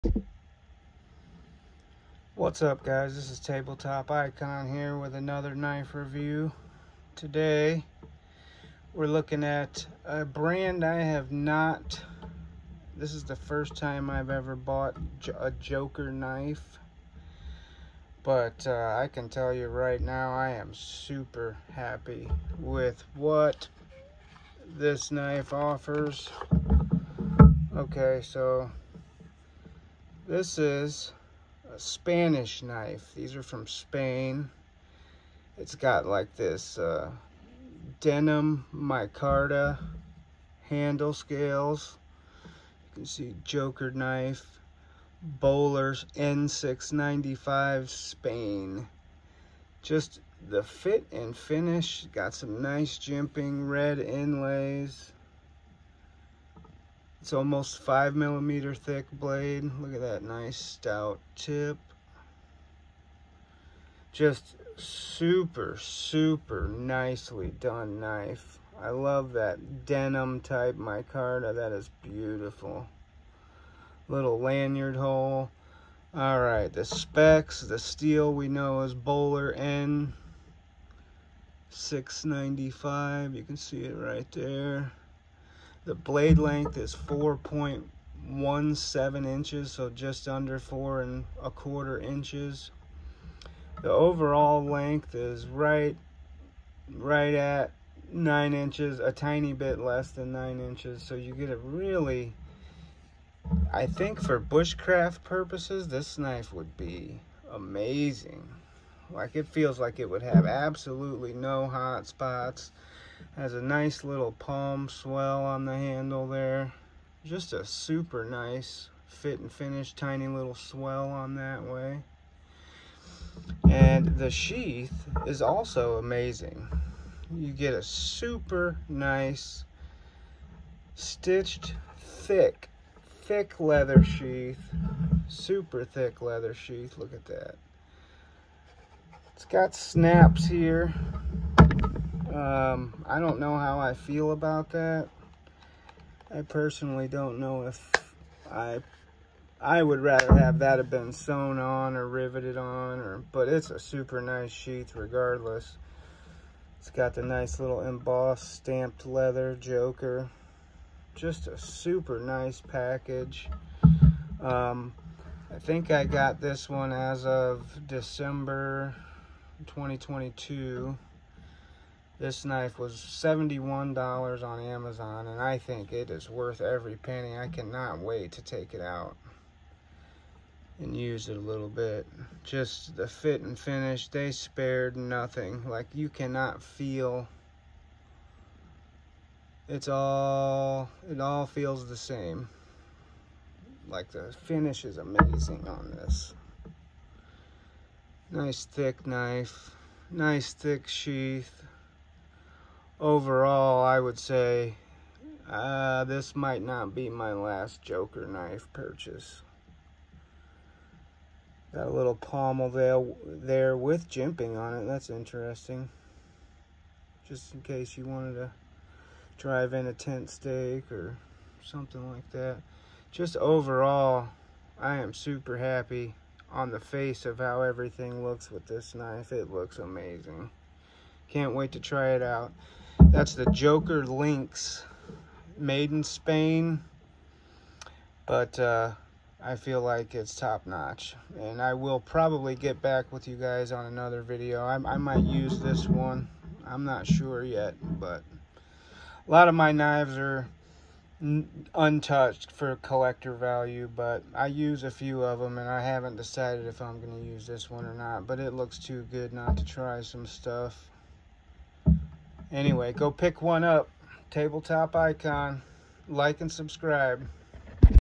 The Joker Lynx fixed blade!! A tabletop up close review!!
Joker Lynx fixed blade is amazingly crafted and is a beautiful knife!! Tabletop review!! Check it out on Amazon by clicking below